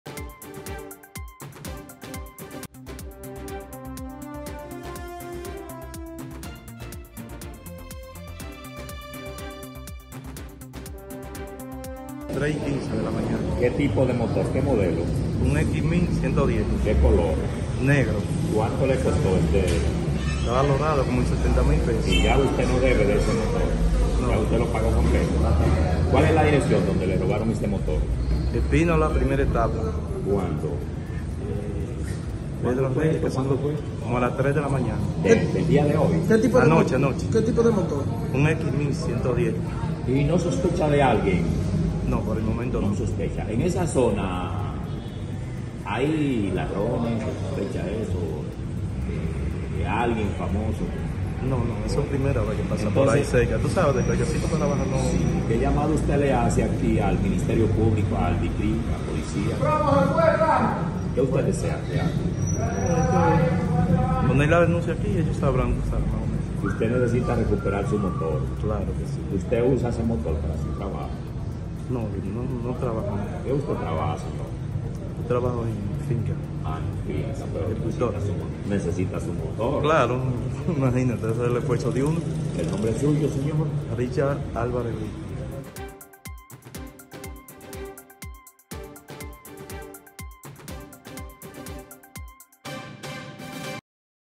Hombre denuncia que desconocido roban su motocicleta en SFM
SAN FRANCISCO DE MACORIS.- Un hombre denunció que desconocidos robaron su motocicleta parqueada en su vivienda del sector Los Espinola primera etapa en esta ciudad.

Richard Álvarez Brito, expresó que aprovecharon que se encontraba durmiendo para sustraer la motocicleta 110, x1000, de color negro, valorada en unos 75,000 mil pesos.

Pidió a las autoridades investigar el caso con la intención de recuperar su motocicleta.